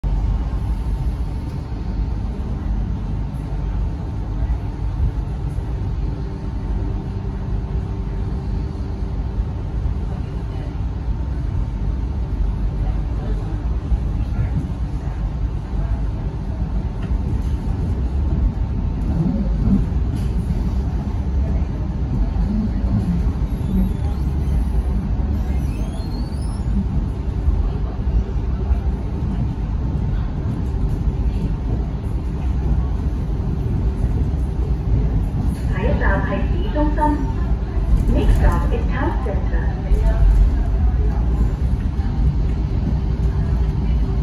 港鐵廣播：市中心（輕鐵路綫614P往屯門碼頭方向）（1142 + 1152）（1152）
（粵）下一站係市中心。
（英）Next stop is Town Centre.